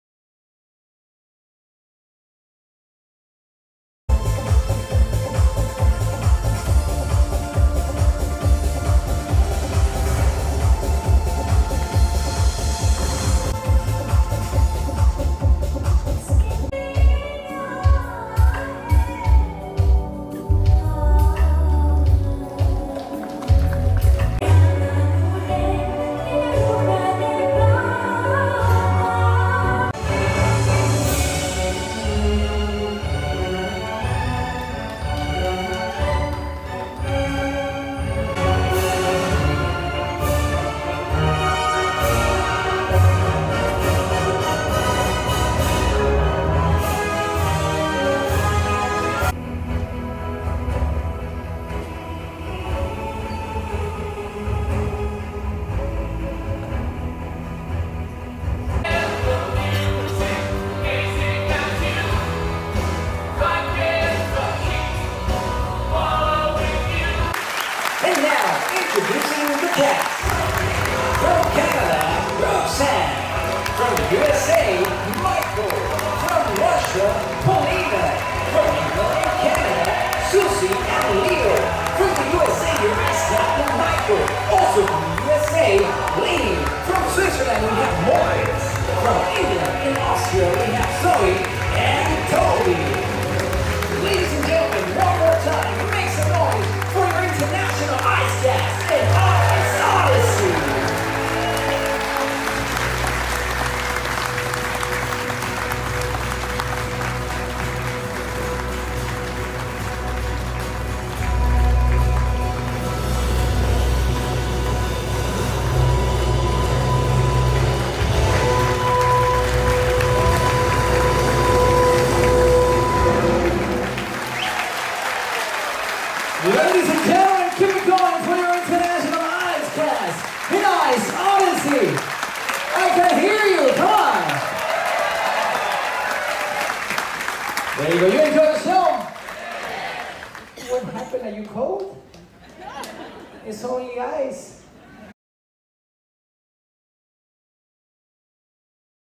Royal Caribbean Voyager of the Seas Ice Skating Show April 19, 2018
"Ice Odyssey" onboard Voyager of the Seas